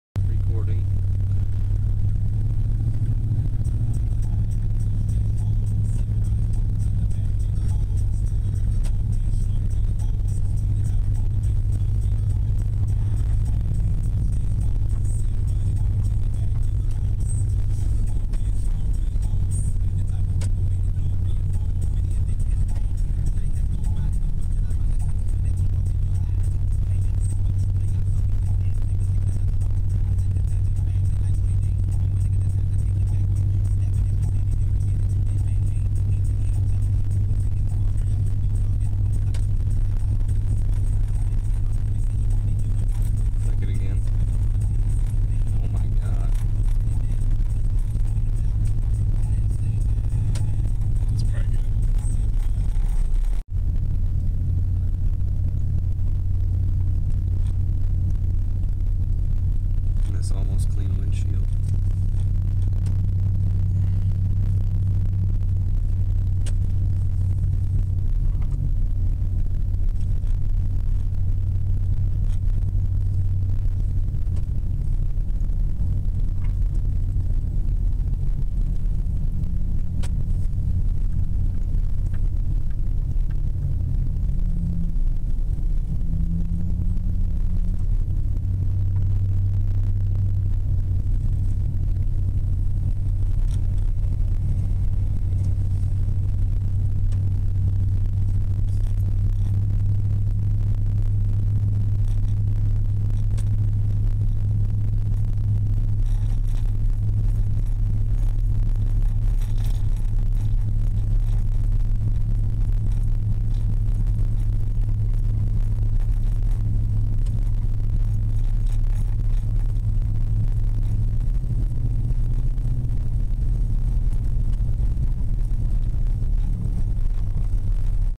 2010 Dodge Ram 1500 - Factory Lights Replaced with Bi-Xenon HID
Factory Lights first, HID lights start at 53s.